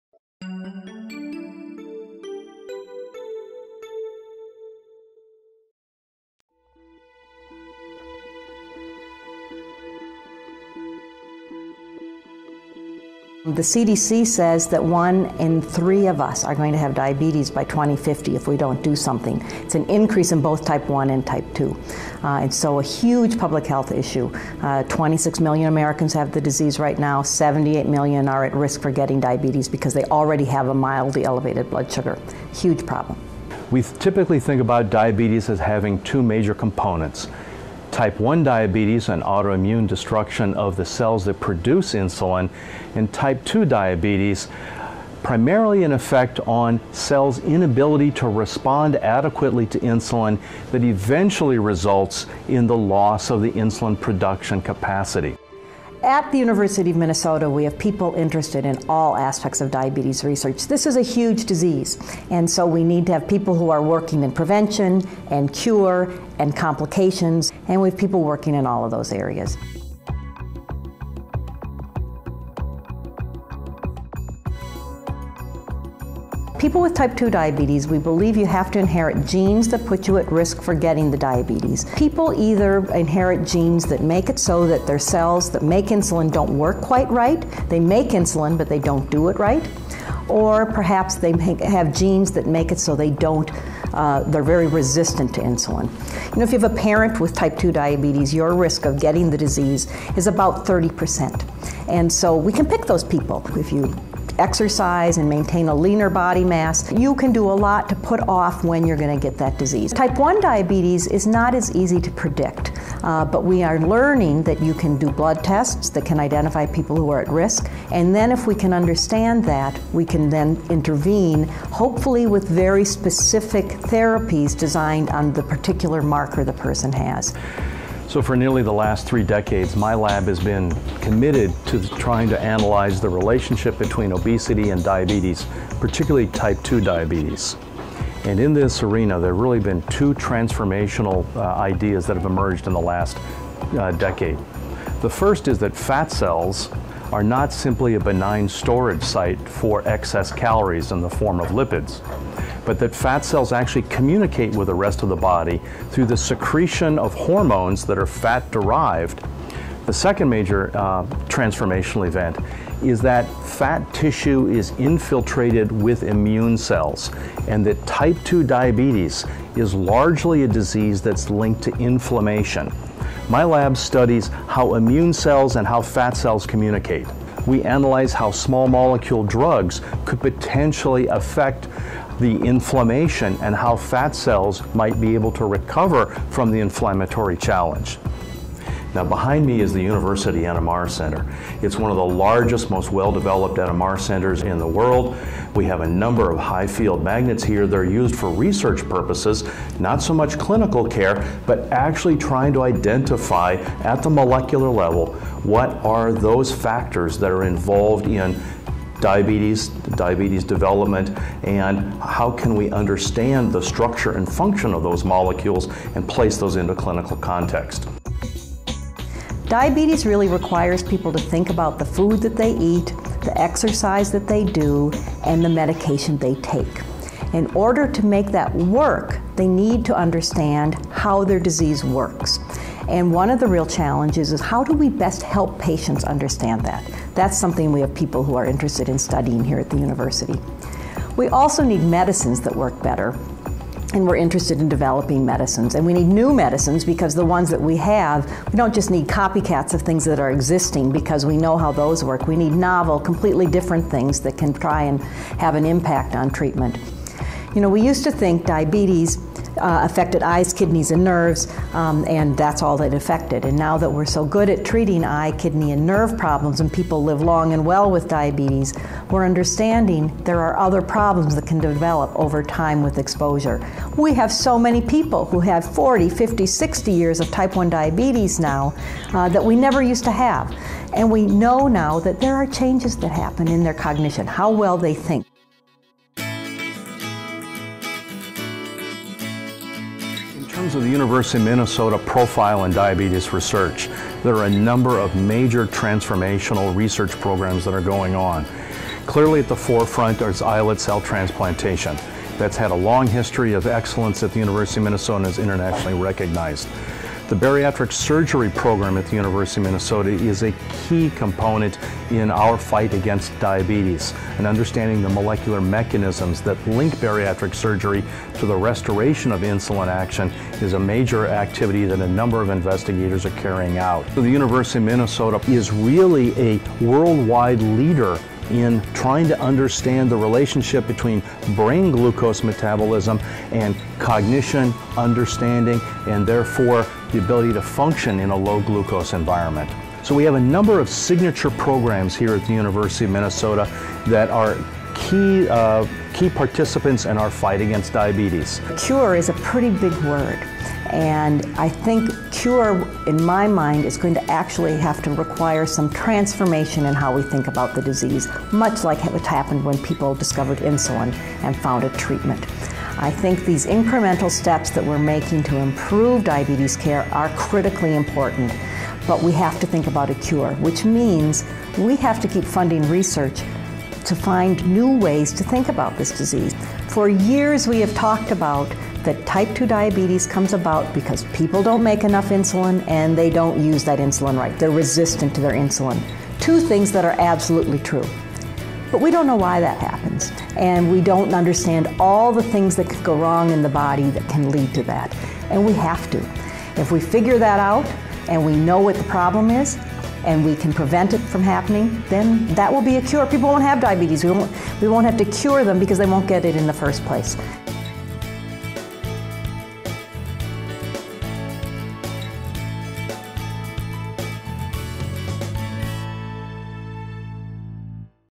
DIABETES RESEARCH CREATING HOPE
The University of Minnesota is leading the charge in developing a worldwide cure for diabetes.